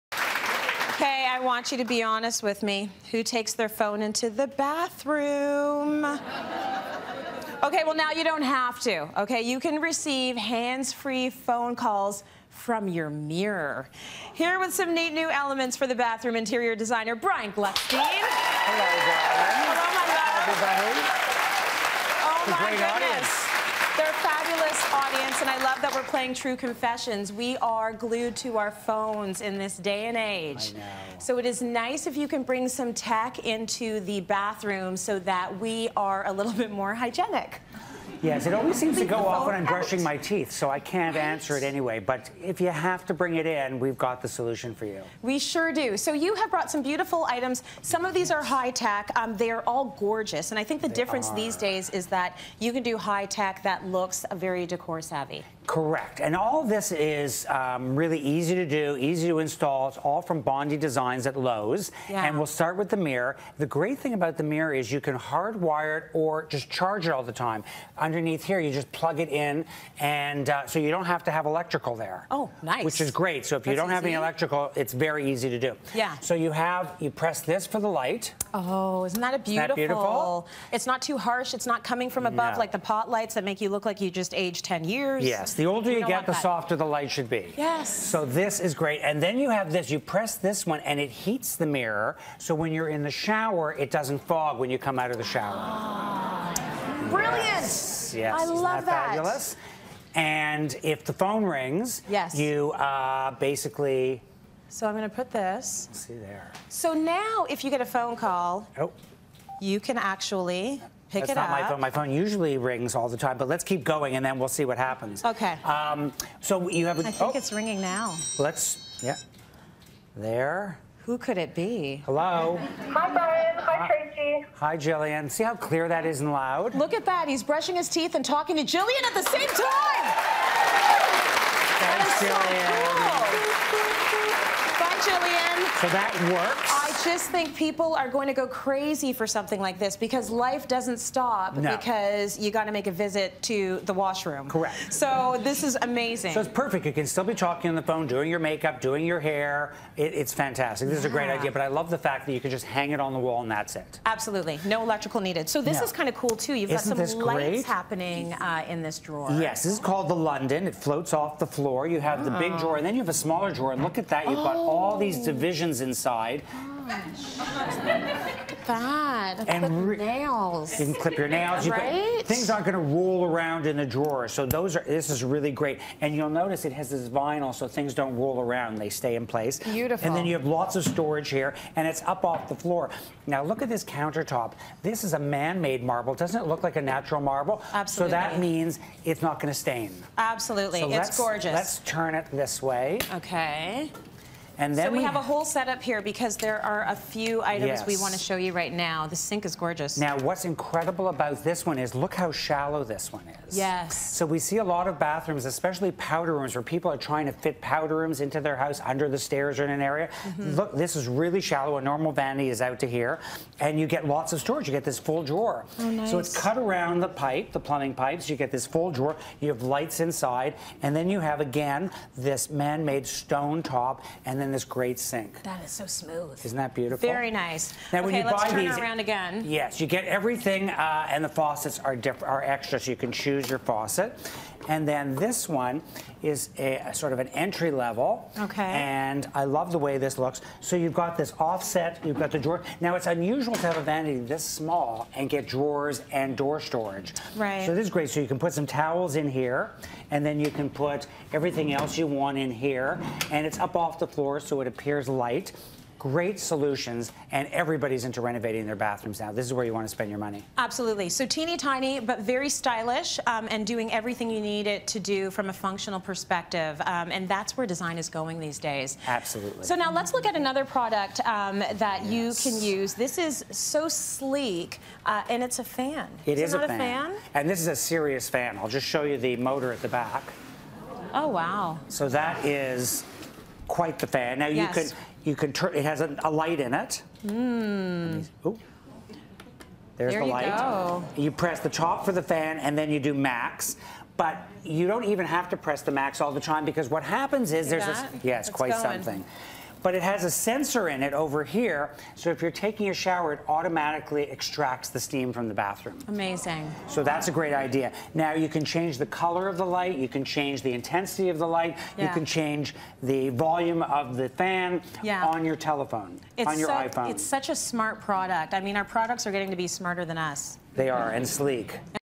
5 beautiful high-tech bathroom upgrades
Designer, Brian Gluckstein, has the solution for those who bring their phones just about everywhere. Upgrade your bathroom with a high tech mirror that can make calls and doesn't fog when you take a shower!